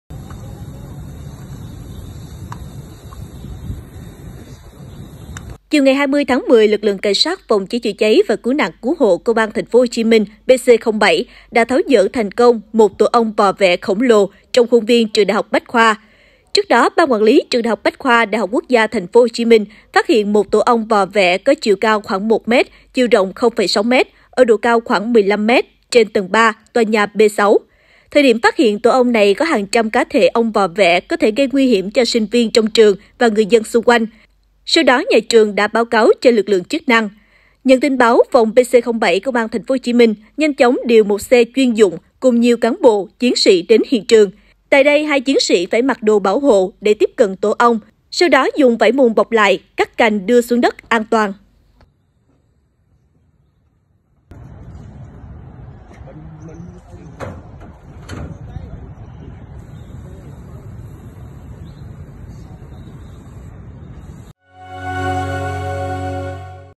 Cảnh sát bắt tổ ong vò vẽ cao 1m ở trường Đại học Bách khoa TP.HCM
Cảnh sát PCCC và CNCH Công an TP.HCM (PC07) đã tháo dỡ thành công một tổ ong vò vẽ khổng lồ có chiều cao khoảng 1m, chiều rộng 0,6 m trong khuôn viên Trường Đại học Bách khoa - Đại học Quốc gia TP.HCM.